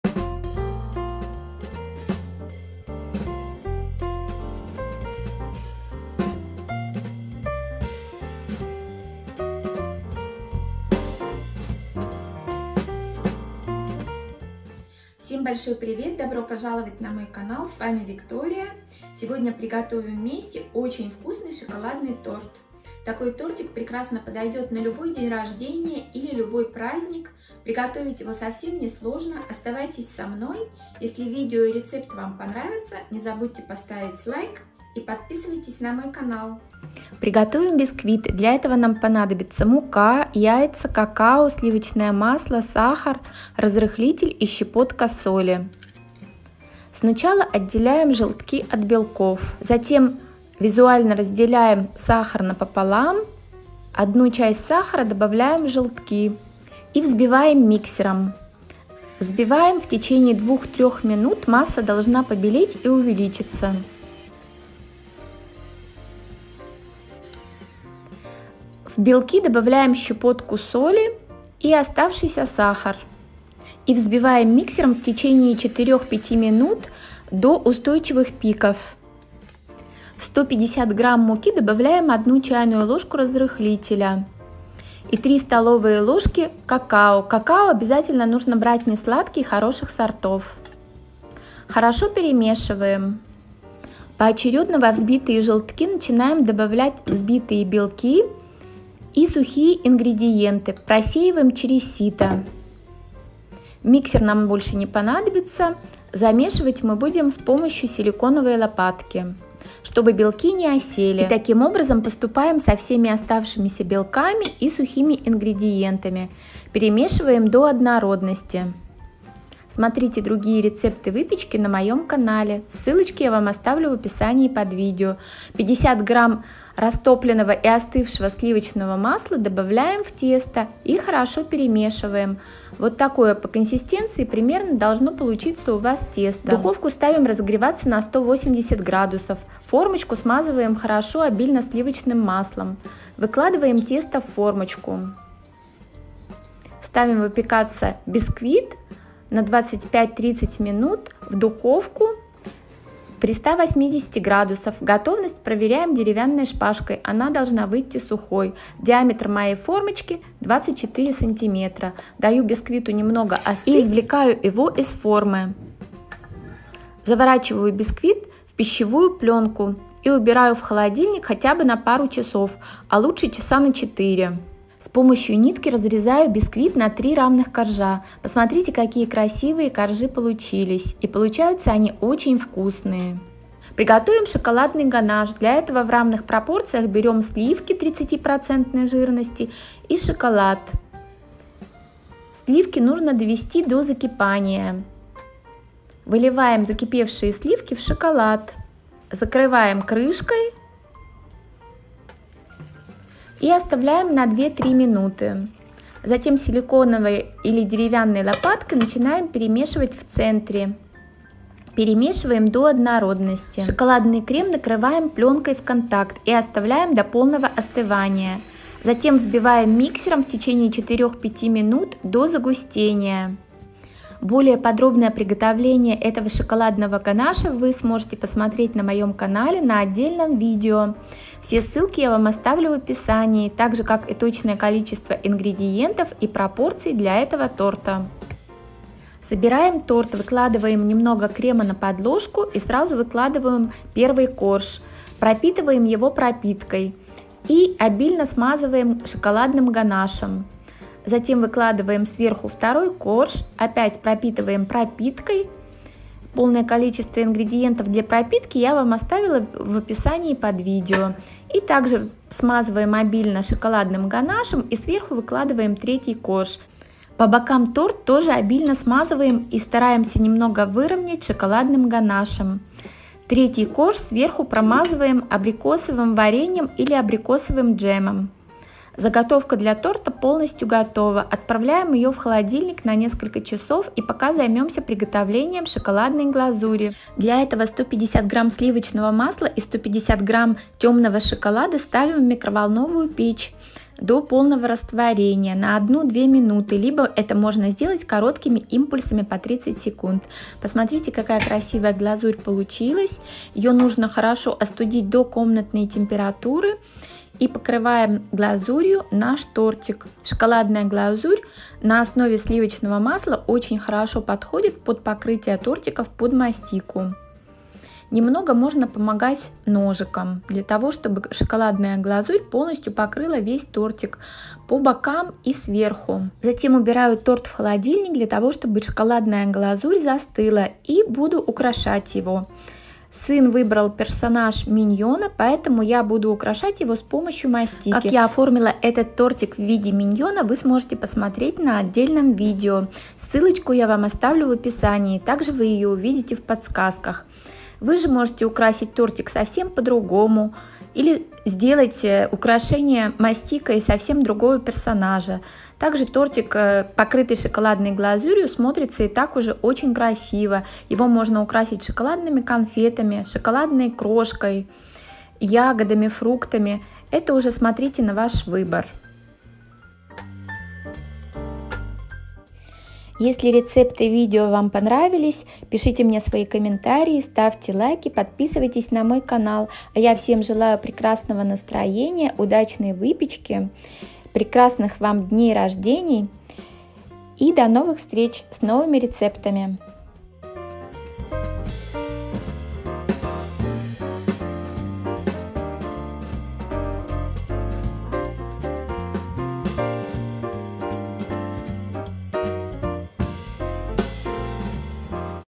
Шоколадный Торт! Вкусный Торт На День Рождения !!! 🍰🎂 🍰 Кулинарим с Викторией.
Друзья, сегодня готовим Шоколадный Торт - ВКУСНЫЙ ТОРТ На День Рождения.
Шоколадный бисквит в сочетании с Шоколадным ганашем и Шоколадной глазурью - отличная Выпечка на Праздник !

Шоколадный Торт ☆ Торт На День Рождения

Ингредиенты :
🔺Для бисквита:
Какао-порошок (не сладкий)- 3 ст л

Сливки - 300 мл (30 % жирности)
Шоколад - 300 г (черный)

🔺Для глазури:
Шоколад черный - 150 г

🔺Для Пропитки  :
Экстракт ванили (или коньяк или ром ) - 1 ст. ложка
Добавляем в 140 мл воды сахар, перемешиваем и оставляем на небольшом огне до закипания , кипятим 5 минут , остужаем и добавляем 1 ст л экстракта ванили .
На промазку - Абрикосовое варенье (или джем) - 2 ст л